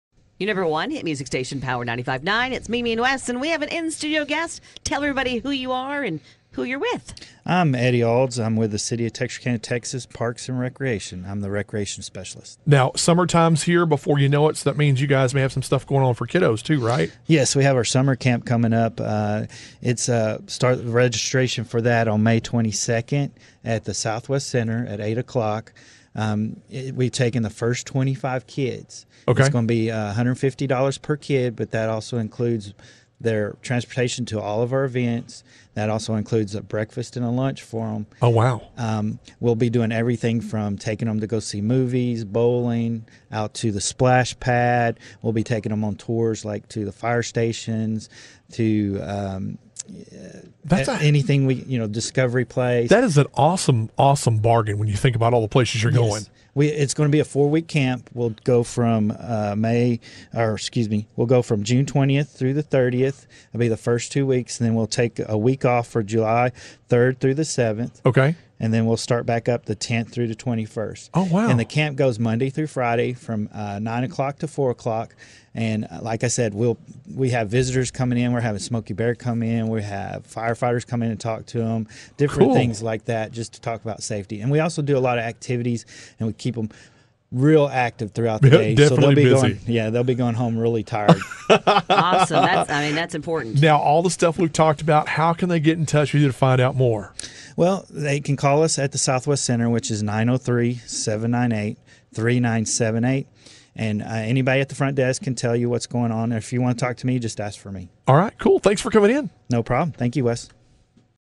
Summer Camp in Texarkana Limited to 25 Kids on FCFS Basis
Find out everything that the Texarkana, Texas Parks & Rec Department has to offer during their Summer Camp